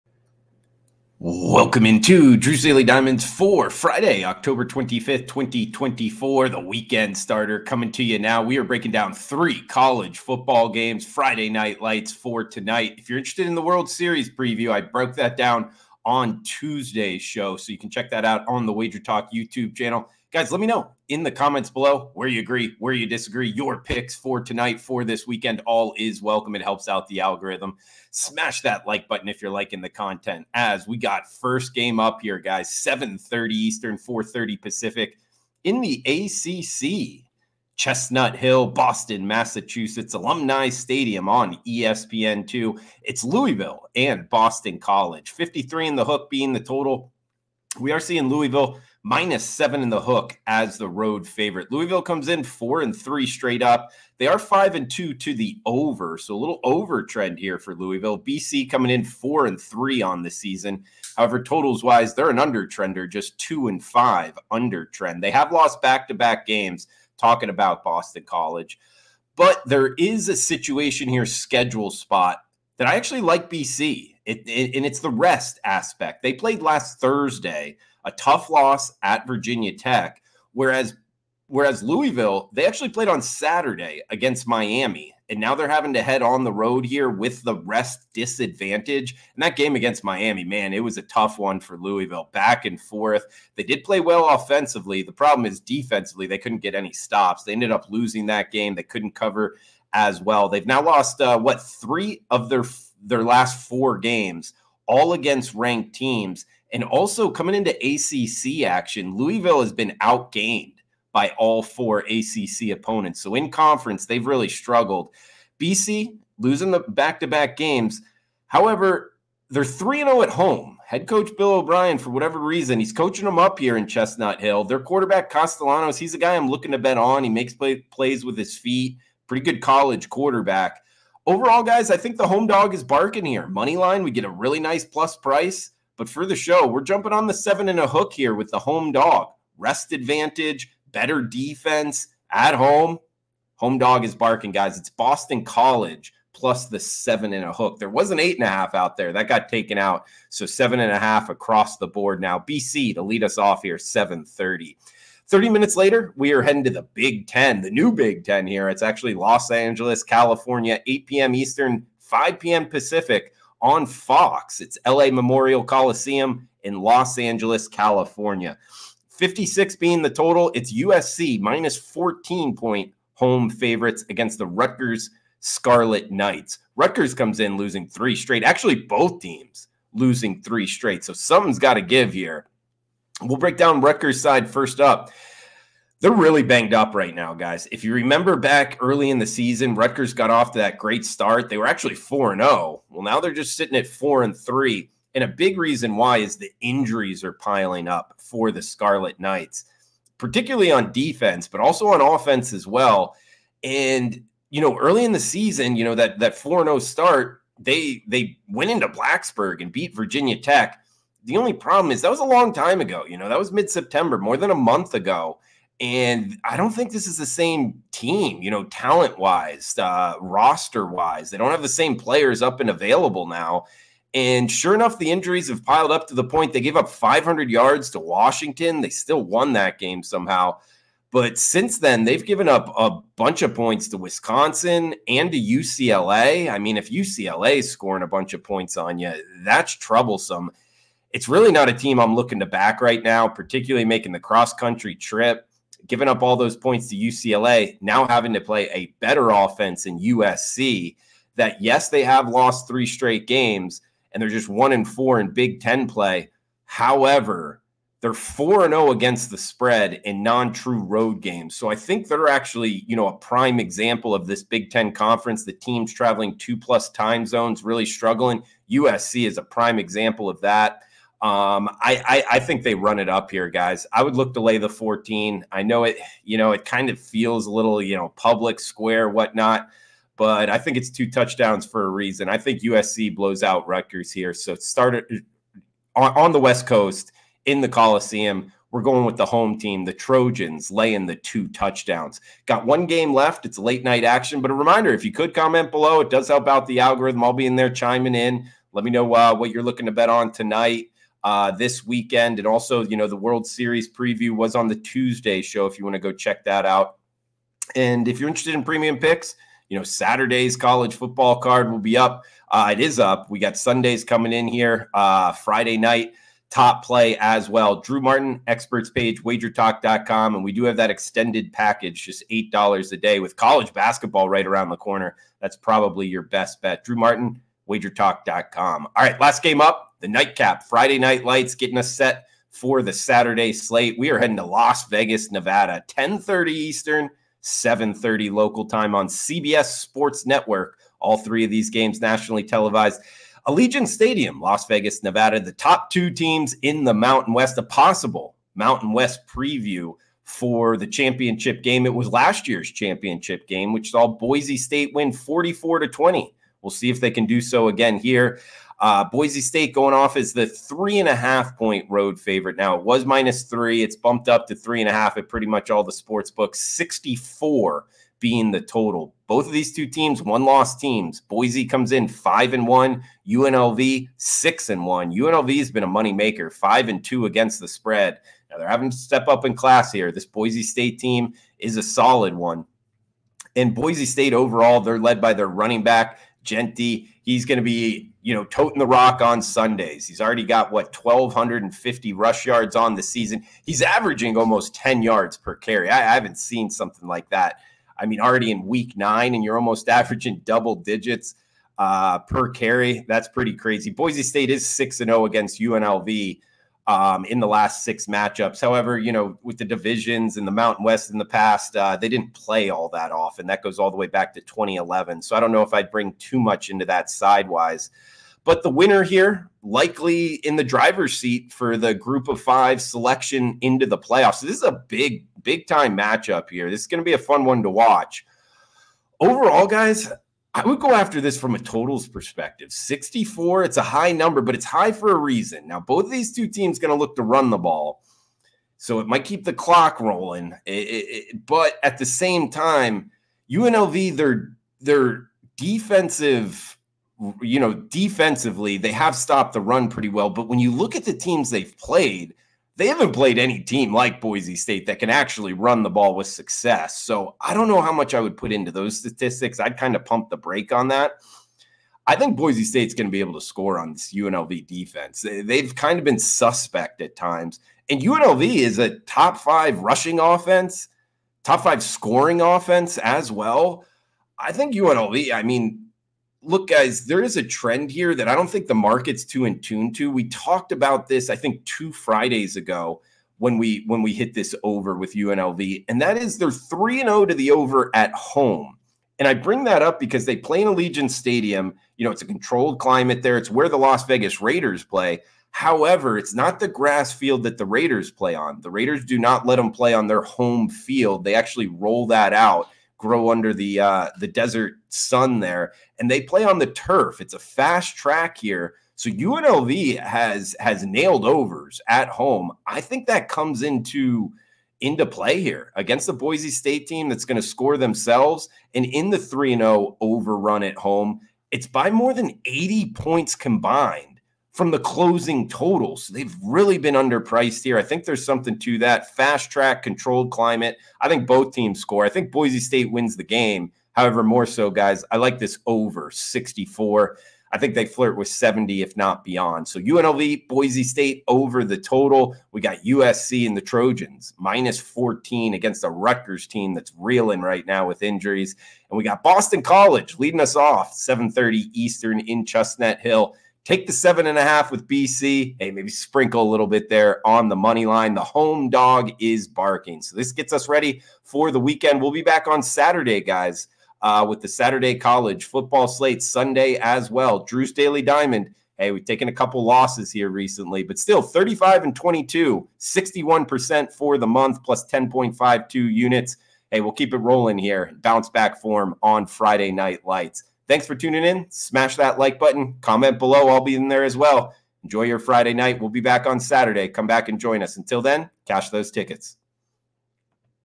Friday College Football Picks and Predictions Today | Drew's Daily Diamond | 10/25/24
Friday College Football Picks and Predictions Today | Drew's Daily Diamond | 10/25/24: Get Friday college football predictions, picks, and best bets from sports betting handicapper Drew Martin on Drew's Daily Diamond for Louisville vs Boston College, Rutgers vs USC and Boise State vs UNLV! Don't miss out!

Introduction 00:00
Louisville vs Boston College 00:31
Rutgers vs USC 02:34
Drew Martin / WagerTalk Friday Offers! 05:03
Boise State vs UNLV 05:27
Drew's Daily Diamond Friday Recap 09:00